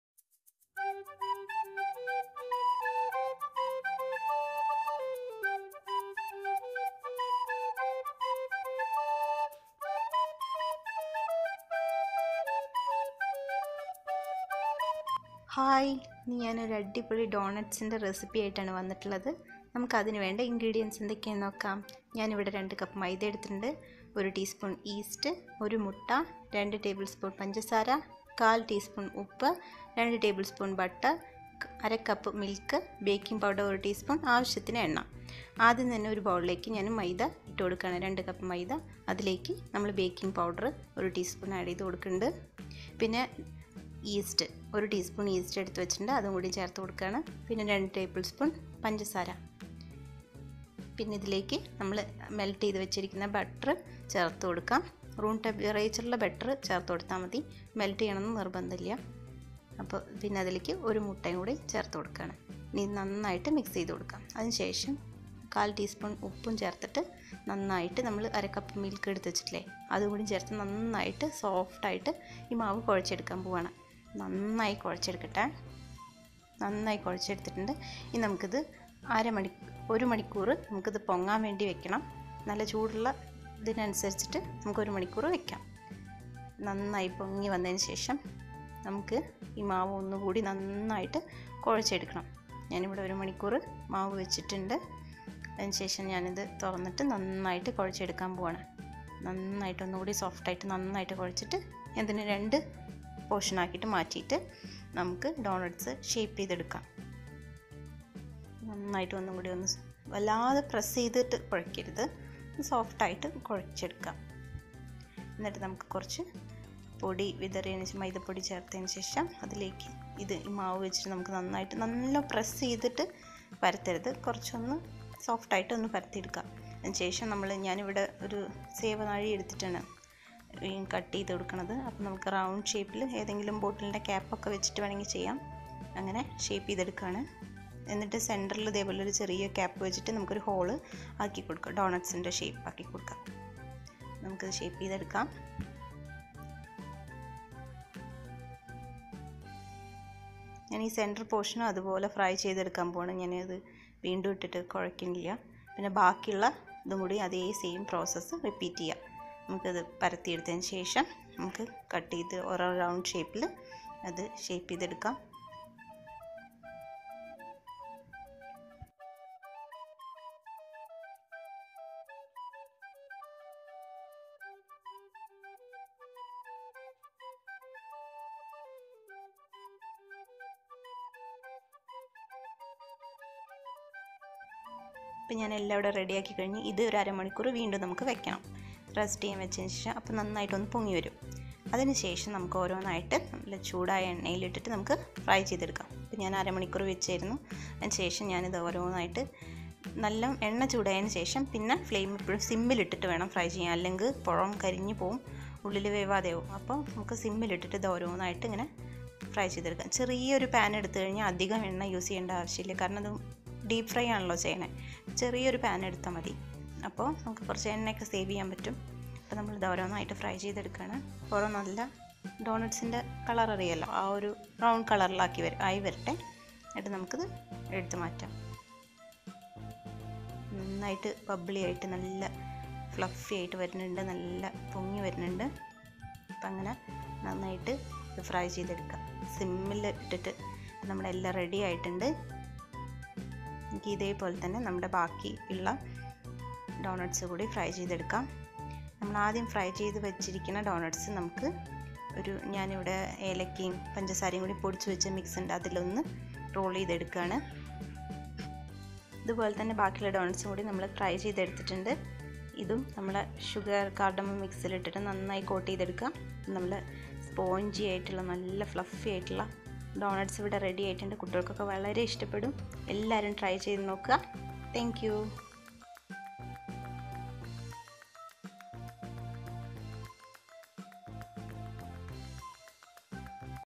Easy spongy doughnuts|| home made doughnuts || kids special
hi... friends..
today you have an easy doughnuts recipe...
Ingredients :-
maida 2cup
yeast 1tsp
egg 1nos
oil
easyhomemade doughnuts